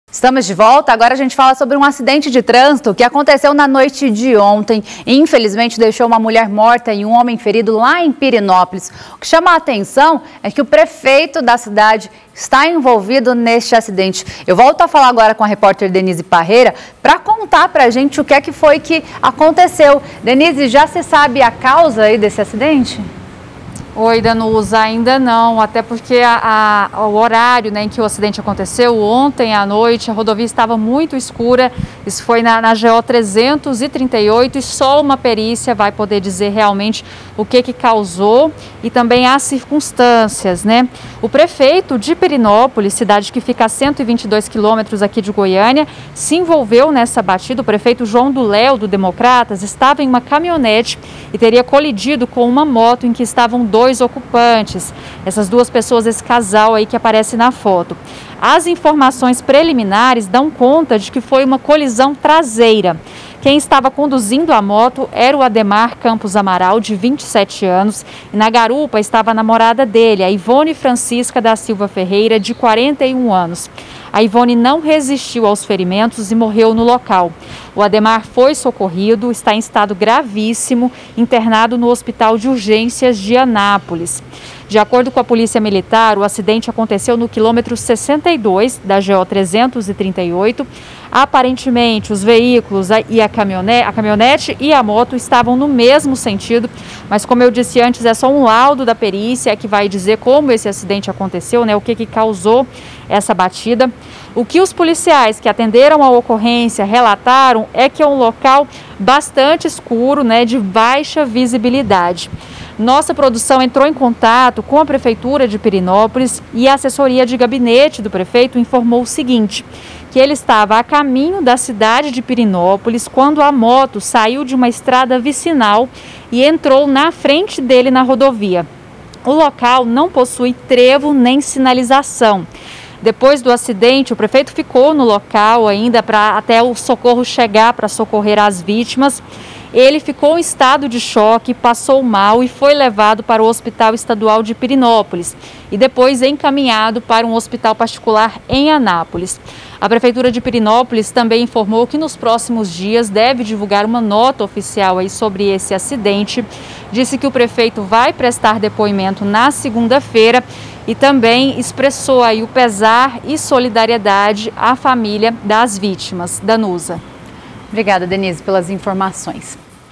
ACIDENTE COM PREFEITO EM PIRENÓPOLIS MATA PASSAGEIRA DO OUTRO VEÍCULO
Data de veiculação: 21/11/2020
Apresentadores: Danuza Azevedo
Repórter: Denyse Parreira